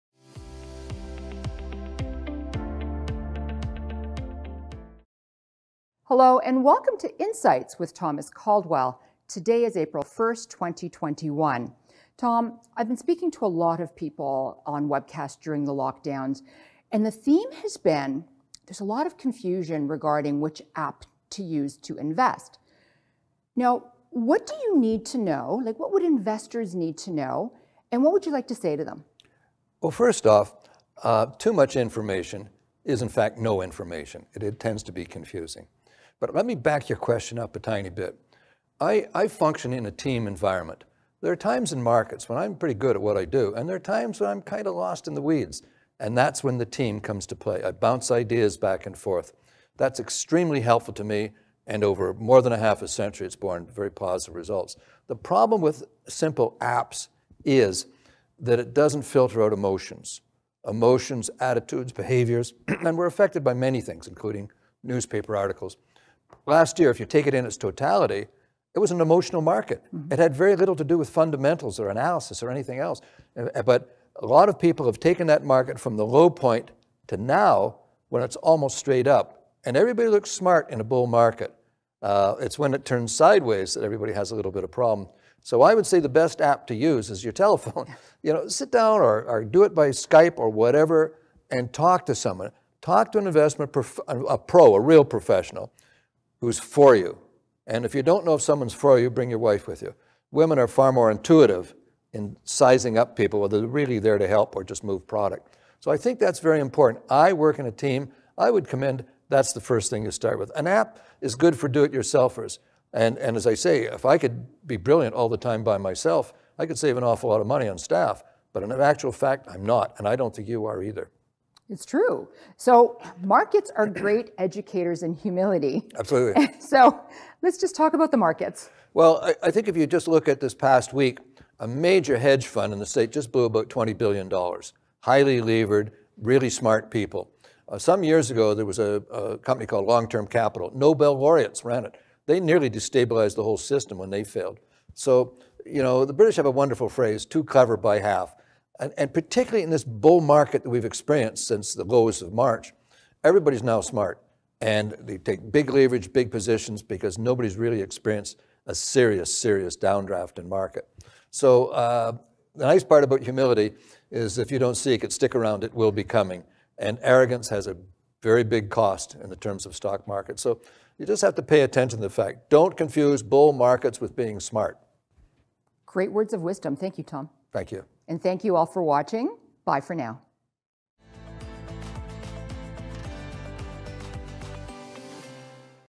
Confusion On Which App to Use For Investing and Markets Are Great Educators of Humility | Episode #2
Welcome to Insights…with Thomas S. Caldwell, C.M. on April 1, 2021.

In this episode of Insights, we cover:

The confusion regarding which app to use for investing and the fact that markets are great educators in humility.

Taped at Vantage Venues